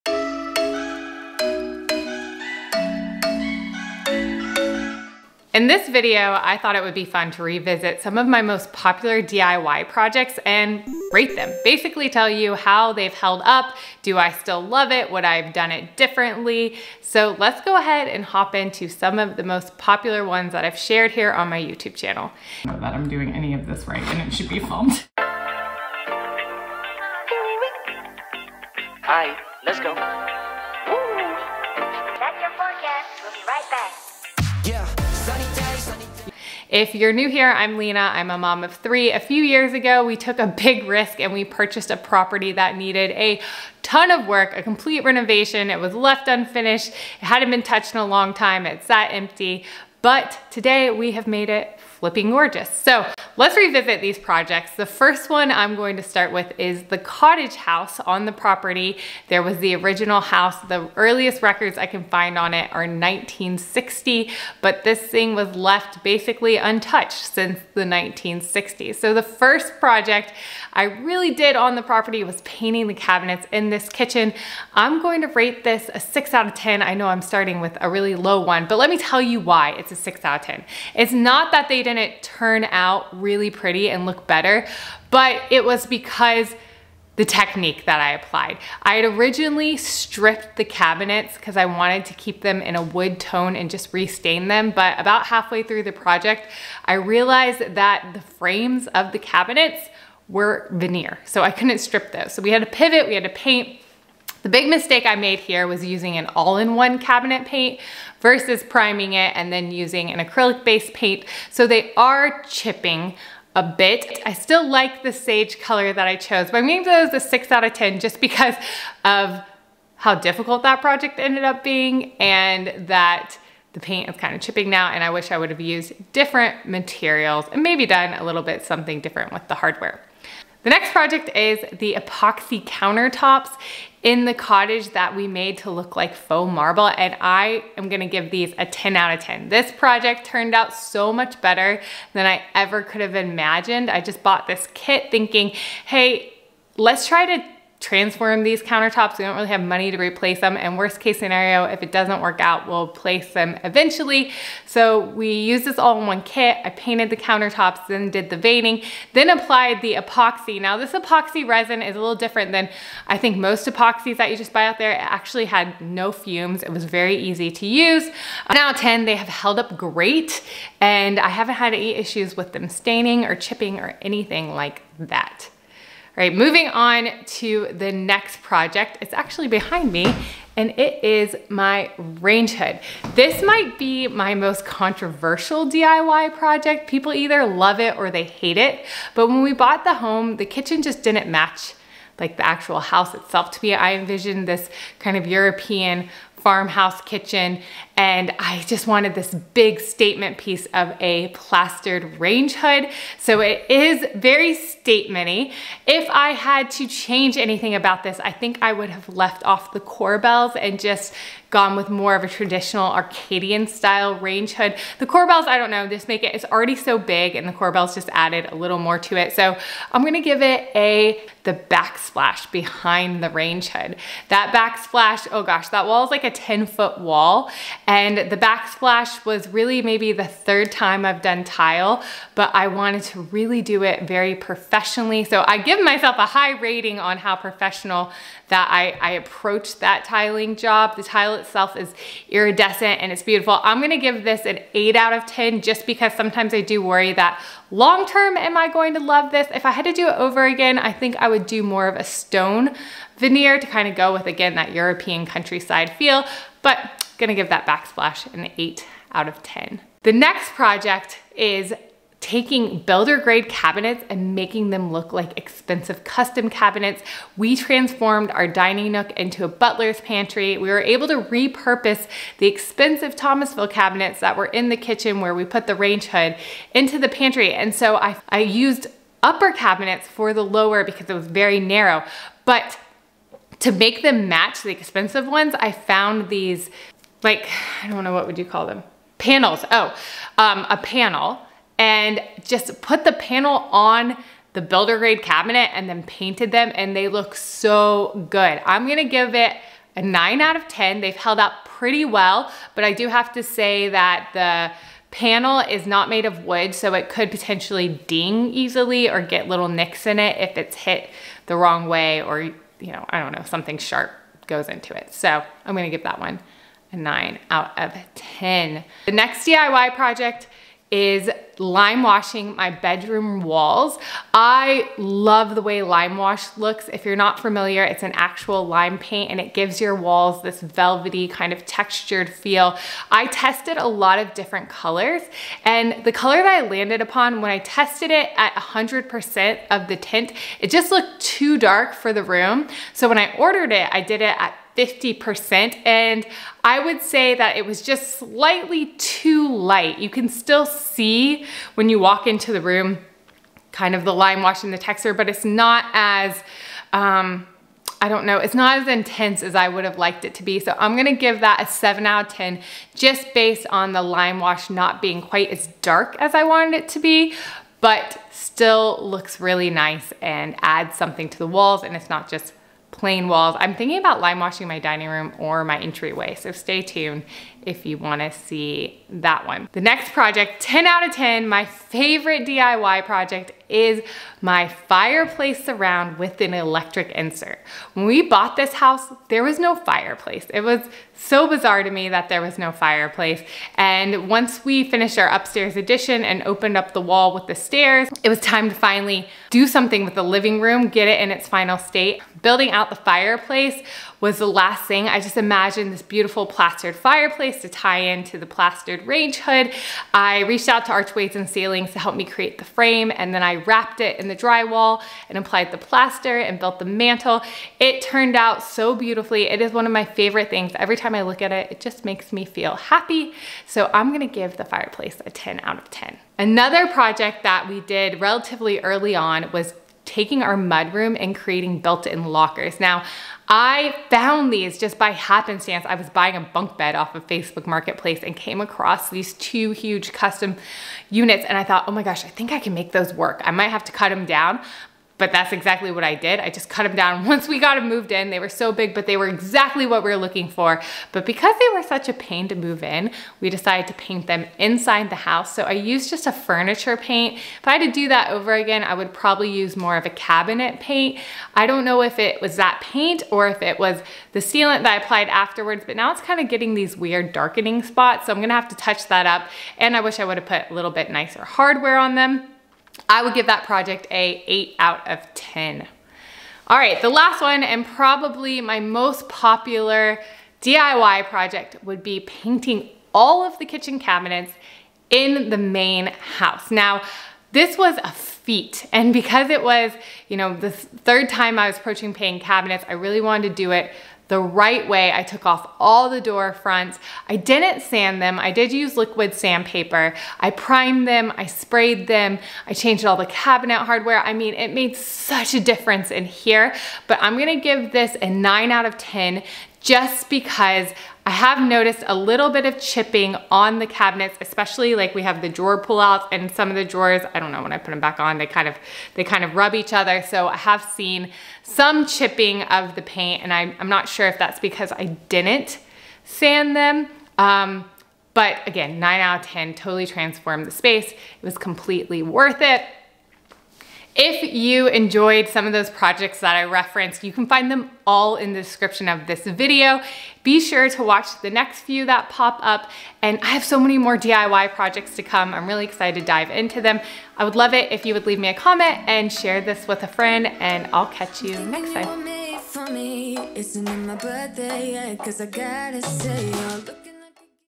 What I’d Do Differently: Revisiting My Favorite DIY Home Projects
In this video I thought it would be fun to revisit my most popular DIY projects that I've done in my home. I'm sharing the good, the bad, and the ugly of taking on these projects and sharing tips on what I would do differently.

📱CONNECT WITH LENA ON SOCIAL